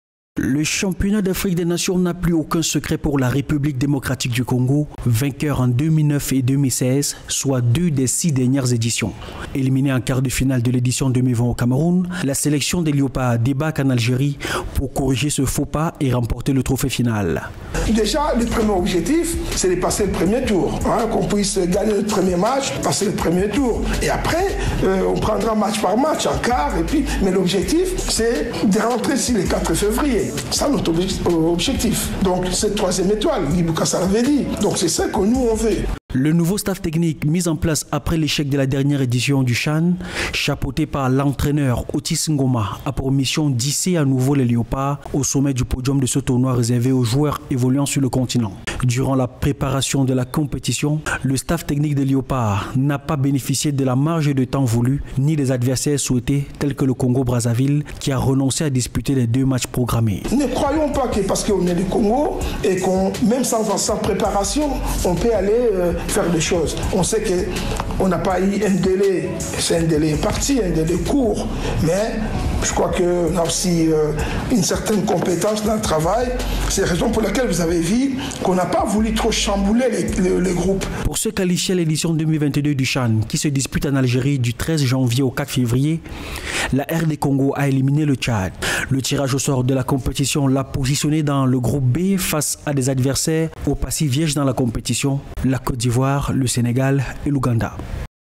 Chan 2022 : la RD Congo en quête d’un 3ème titre de champion africain
Le championnat d'Afrique des nations se dispute du 13 janvier au 04 février 2023 en Algérie. Parmi les équipes attendues, la RD Congo, double vainqueur de la compétition en 2009 et 2016, fait office de favorite. En dépit d’une préparation émaillée de nombreux obstacles, la sélection des Léopards A’ espère remporter un troisième trophée.